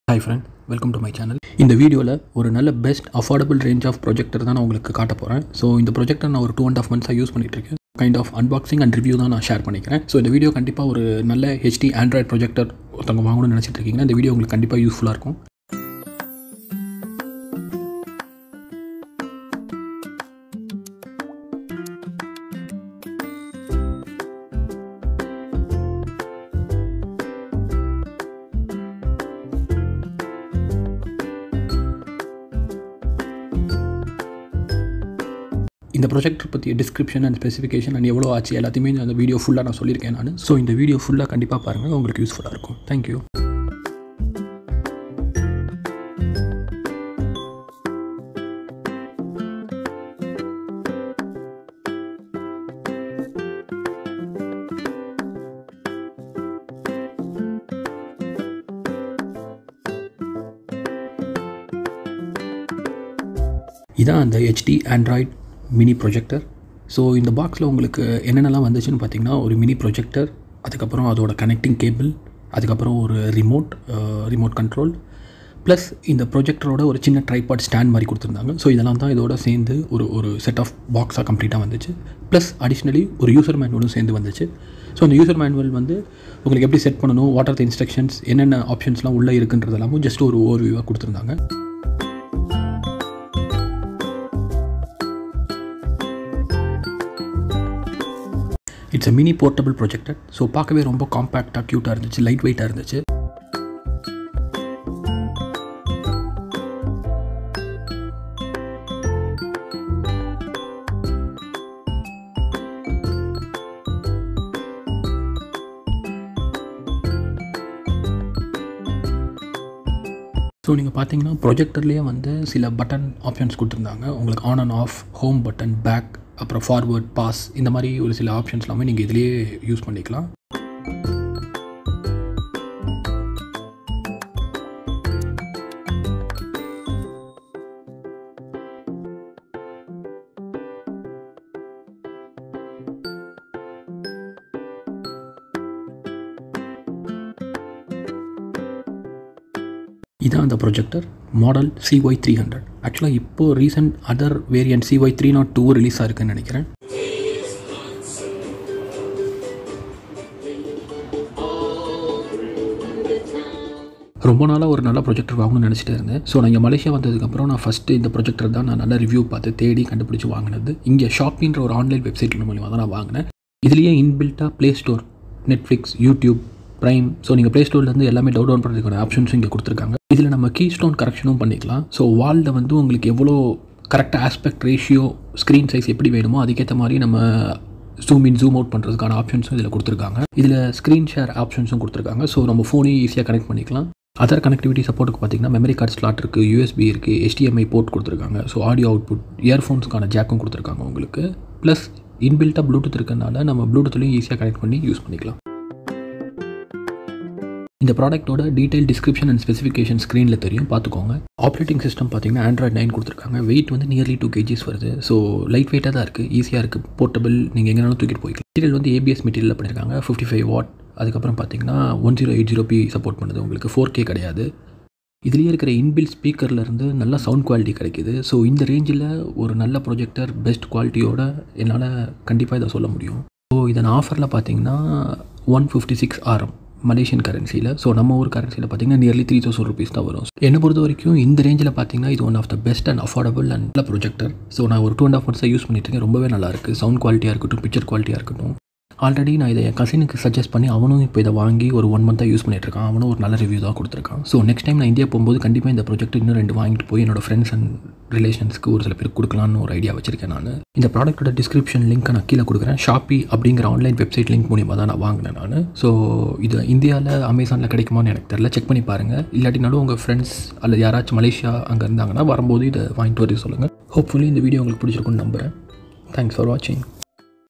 குறைந்த விலை தியேட்டர் | Best Budget Projector | Un boxing | Review | kaushik's hotspot
Cy 300 - Mini Android Portable Projector .

Small and Portable projector Less than 2kg weight, with leather handle can be easy to take. Whether it's indoors or outdoors, make any place your theater.

I hope you guys enjoyed my projector unboxing/review and had something to take away

kaushik.